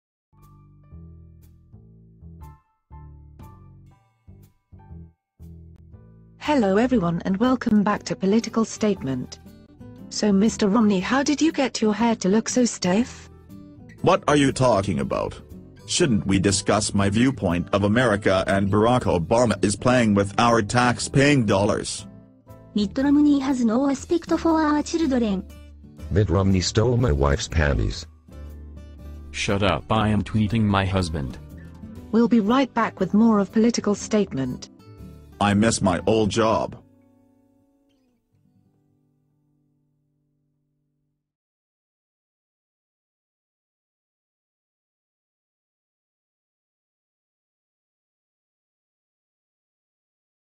Political Statement pt. 1
Join Taylor Evangile as she talks to some of today's top political figures for Election 2012